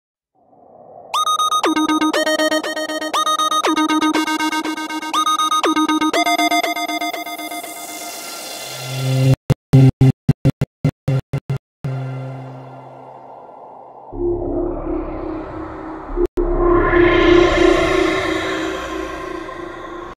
Nooka Extra Dimensional National Video
Nooka watches are pretty cool and come in a range of awesome colours.